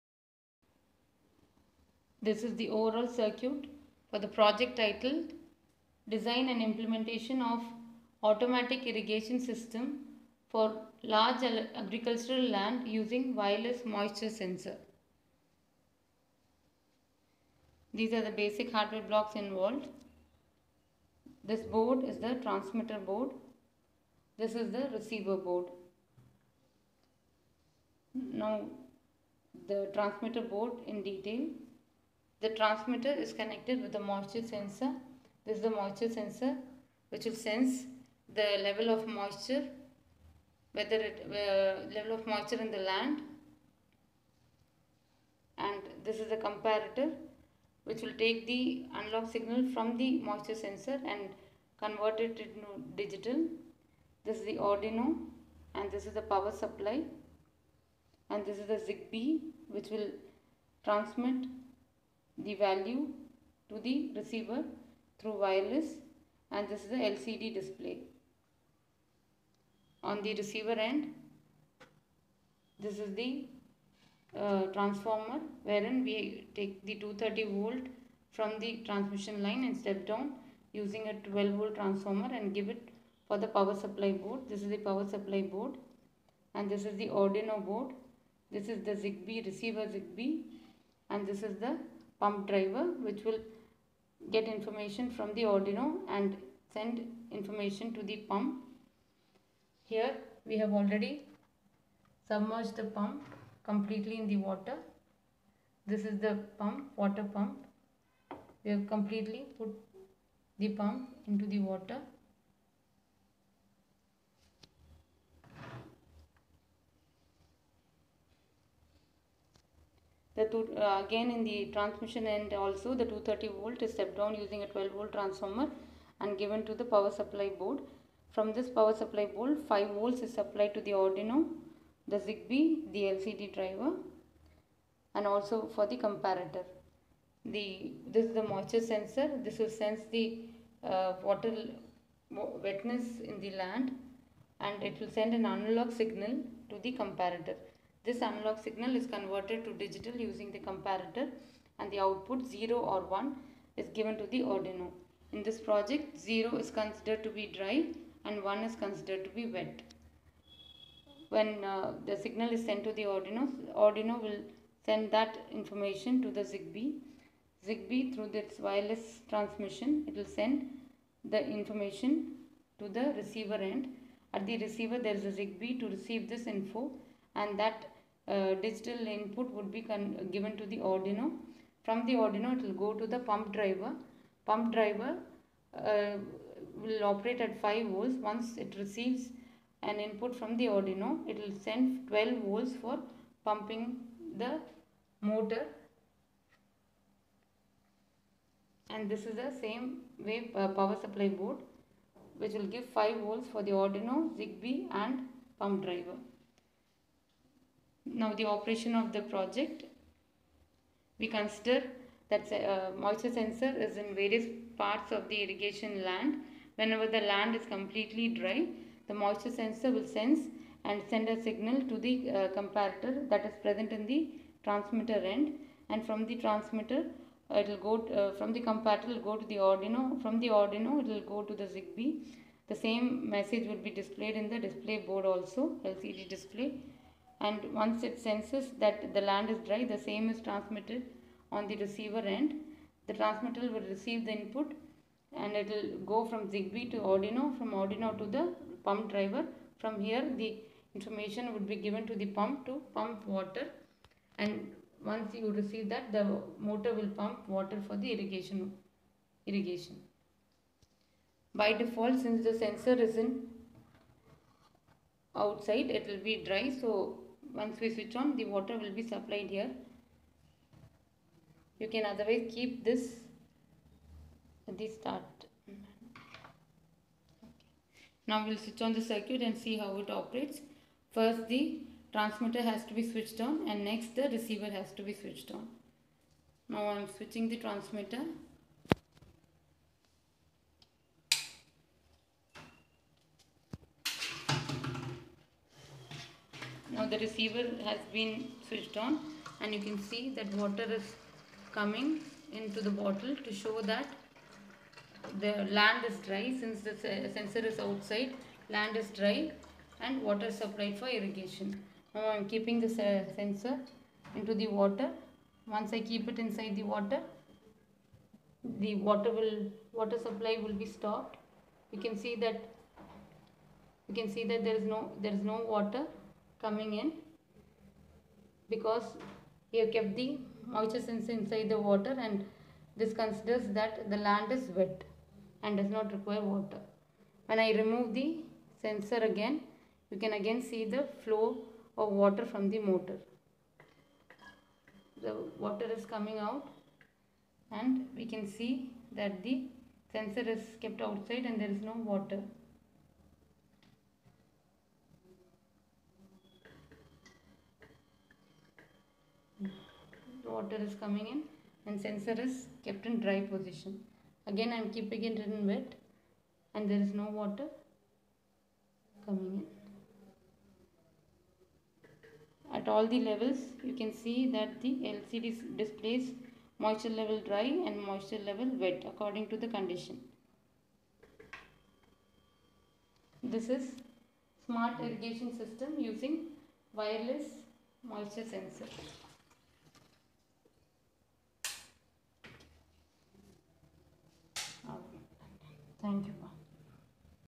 Smart Irrigation Controllers. wireless sensor network for field soil moisture monitoring.
Significance to Industry: High quality irrigation water is becoming increasingly scarce and it is becoming more important to use the available water efficiently. One approach to more efficient irrigation is the use of soil moisture sensors to control irrigation. Soil moisture